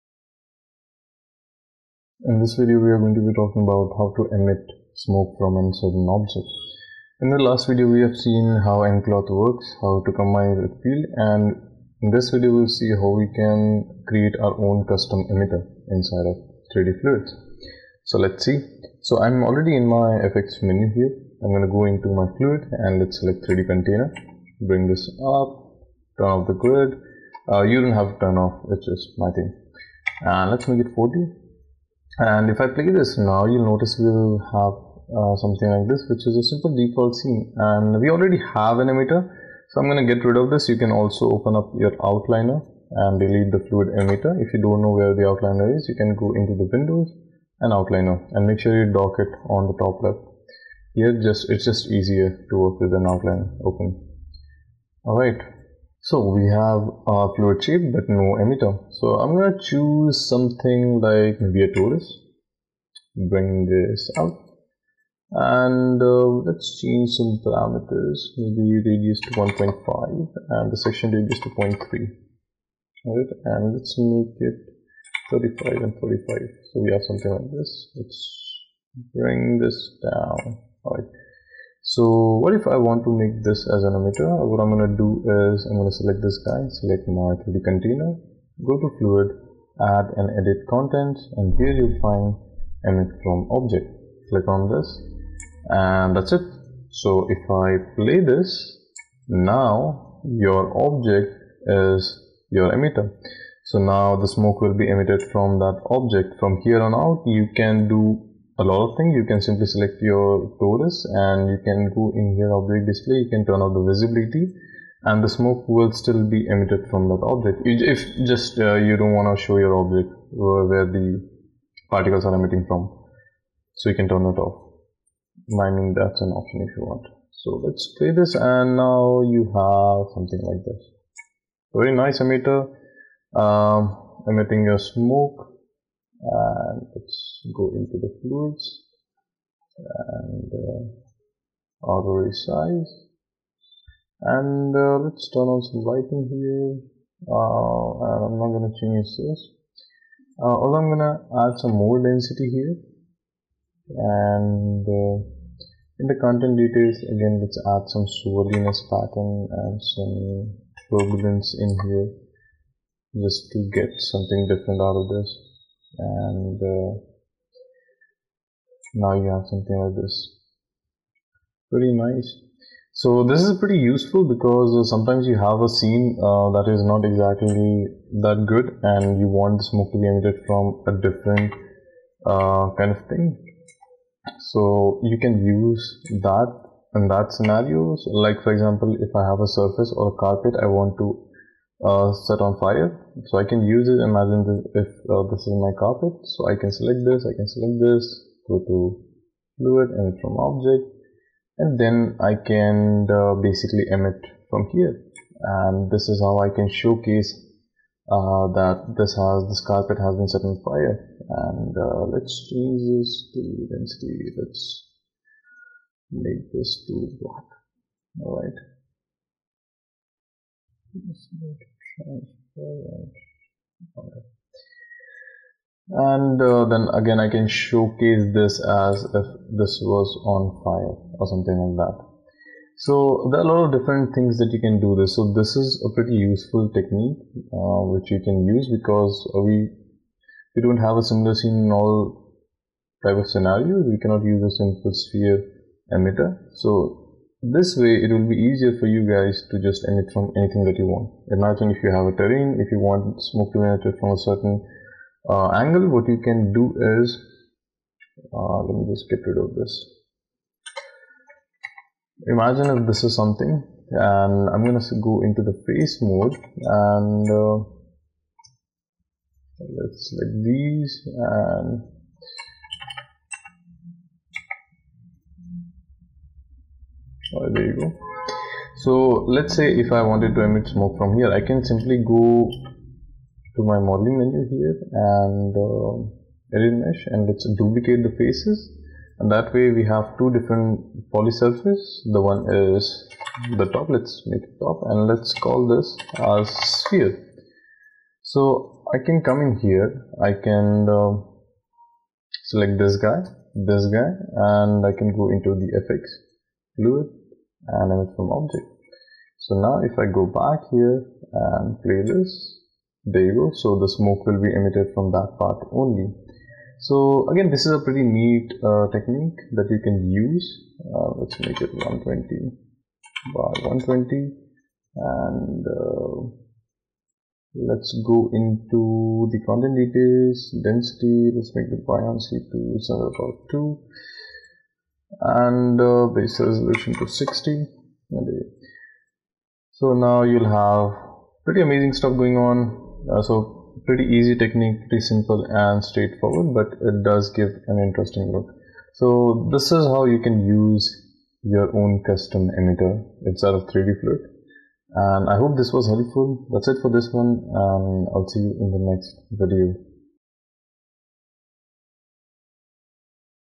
Creating Custom Emitter In 3D Fluids | Maya | Arnold Renderer | Tutorial
Today we are going to learn about creating custom emitters in 3D fluids In Maya And Arnold Renderer.

Behance - Reimagine Fx
Vimeo - Reimagine Fx
Gmail - amarshindebiz